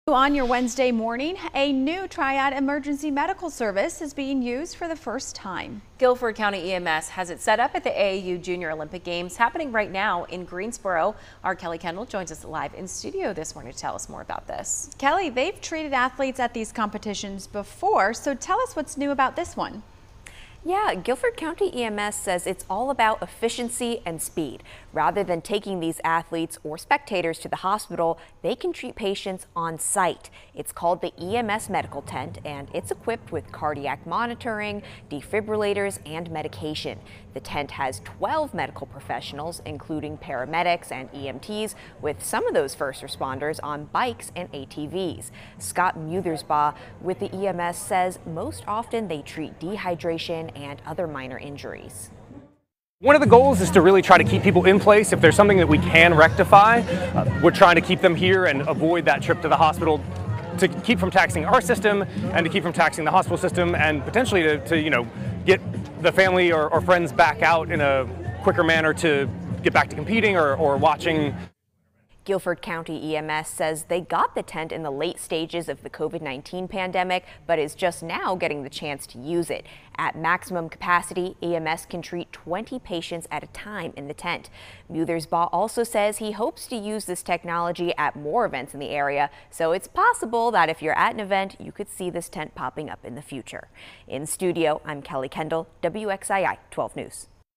Guilford County EMS debuts new medical tent at AAU Junior Olympics
It looks like just a tent on the outside. On the inside, Guilford County EMS is using a new setup to treat young athletes and spectators at the AAU Junior Olympics.